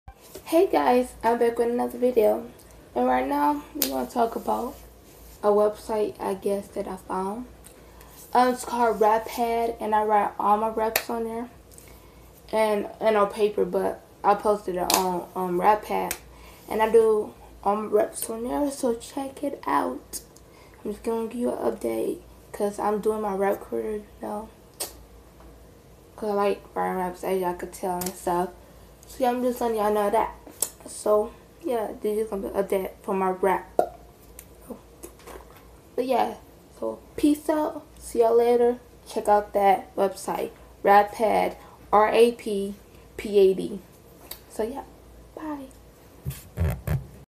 Rappad
Everyone check out my raps on rappad my name on therei s flexiej I'm going to write all my raps.on their and other stuff so look out for  new stuff and this was a update.